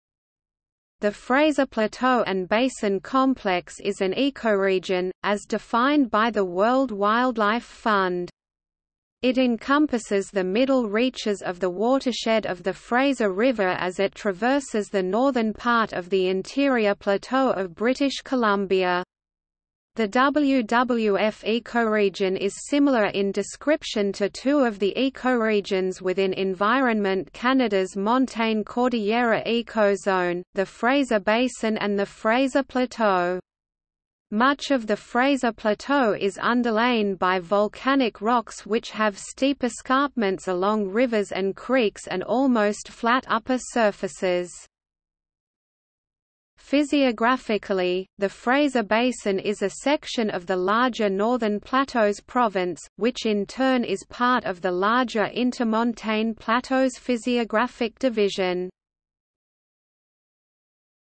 Fraser Plateau and Basin complex | Wikipedia audio article
- Socrates

SUMMARY
The Fraser Plateau and Basin Complex is an ecoregion, as defined by the World Wildlife Fund.  It encompasses the middle reaches of the watershed of the Fraser River as it traverses the northern part of the Interior Plateau of British Columbia.  The WWF ecoregion is similar in description to  two of the ecoregions within Environment Canada's  Montane Cordillera Ecozone: The Fraser Basin and the Fraser Plateau. Much of the Fraser Plateau is underlain by volcanic rocks which have steep escarpments along rivers and creeks and almost flat upper surfaces.
Physiographically, the Fraser Basin is a section of the larger Northern Plateaus province, which in turn is part of the larger Intermontane Plateaus physiographic division.